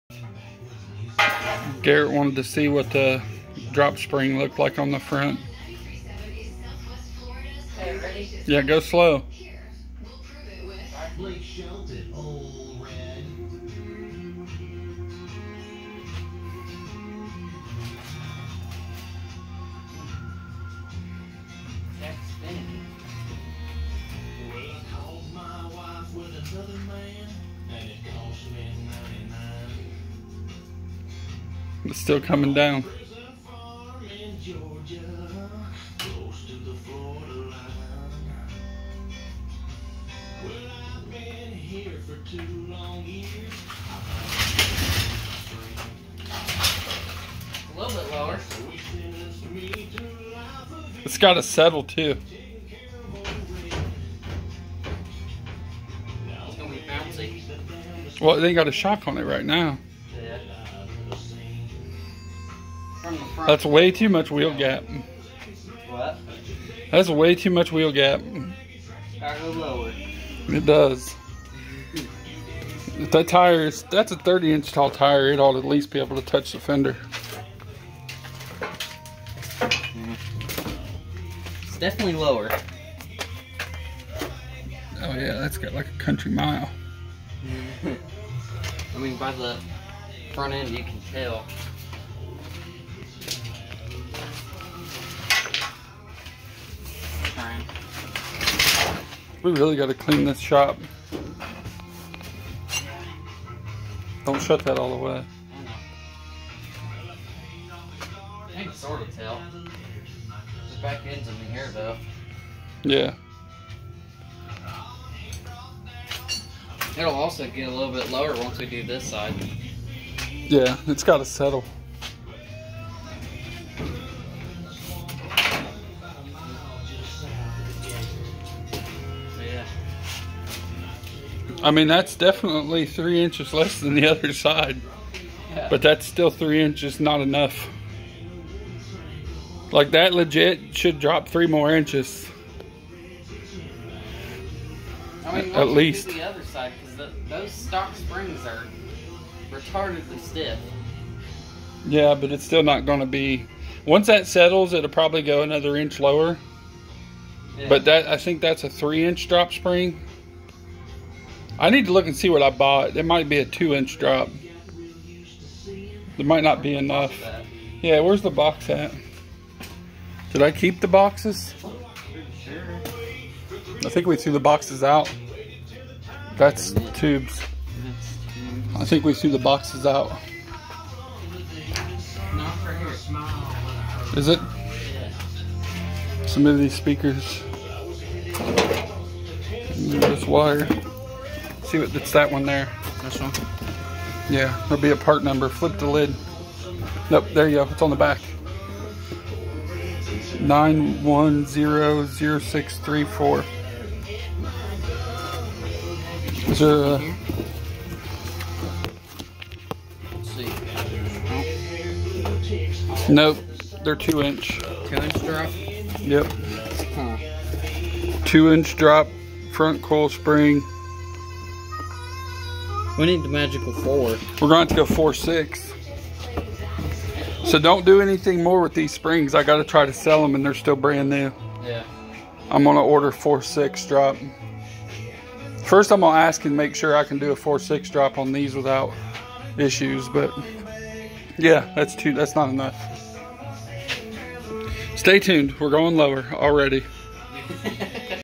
Dropping the 70 c10! Is it low enough? 🤔
Garrett decided to see the c10s drop kit so he installed a front spring. These trucks have 2 miles of wheel gap even with the 30" tall tire. I ordered a 2-4 drop cause I wanted to keep it fun and streetable. It needs air ride asap it's so high even 2" off. I'm going to see if I can sell these new 2 - 4 springs and maybe get 4" 6" springs just for now.